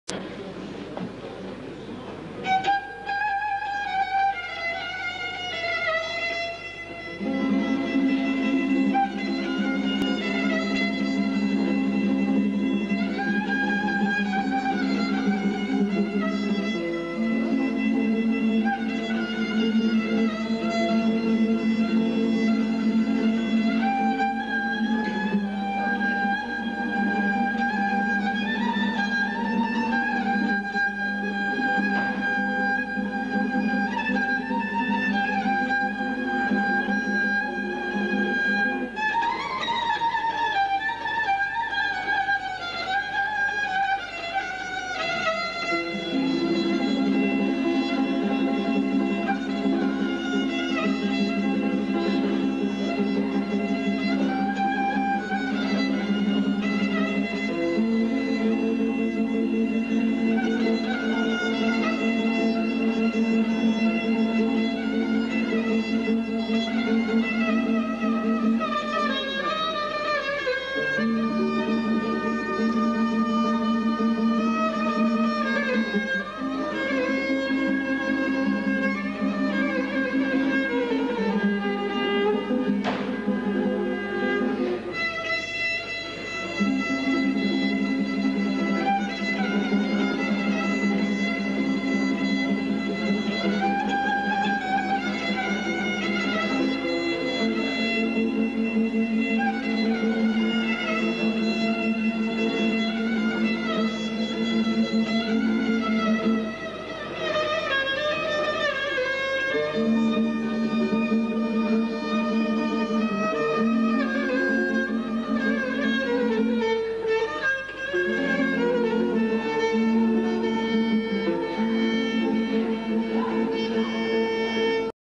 Гъдулка и тамбура - Гълъбово 13.05. 2000 г.
Десети национален събор на гъдуларите в България.